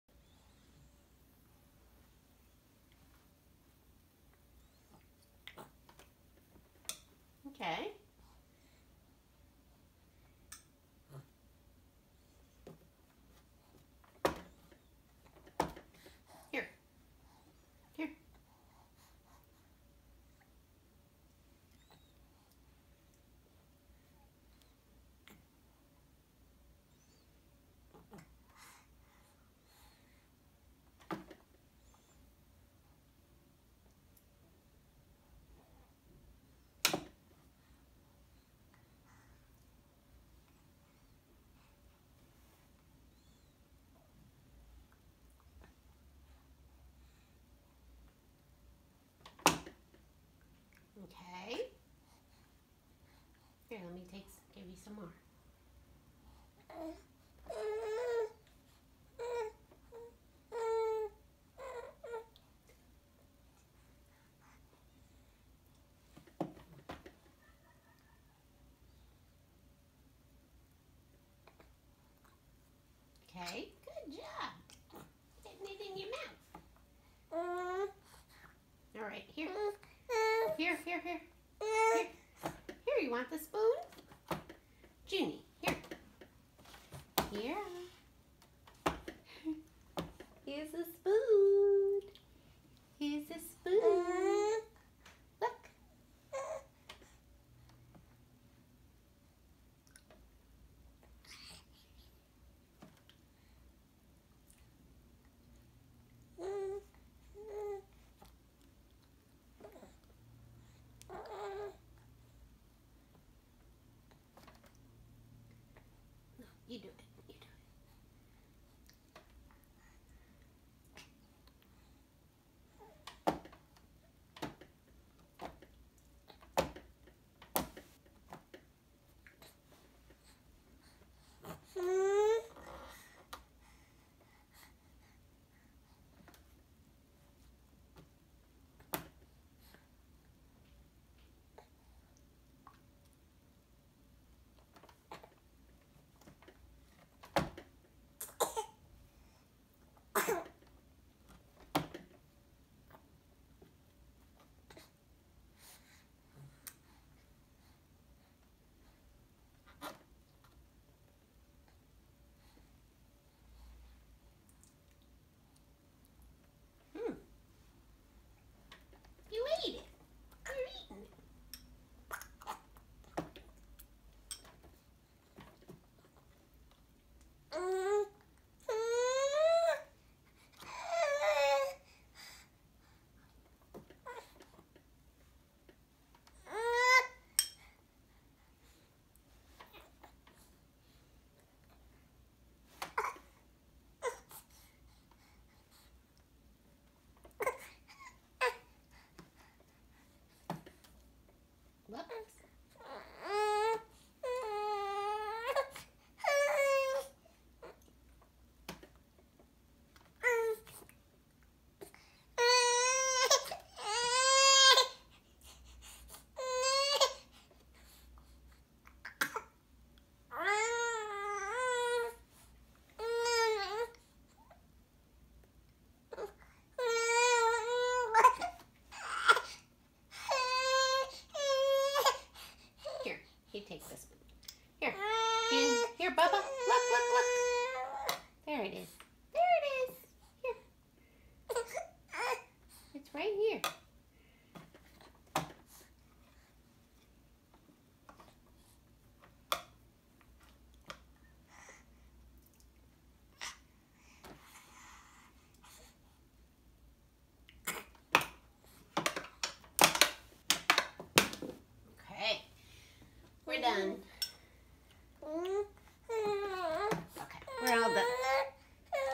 BABY'S FIRST MEAL: Baby eats green beans day 2 - pt. 2
This is the second day in a row the baby is eating her green beans. She really hated them the first day. Will she like them today?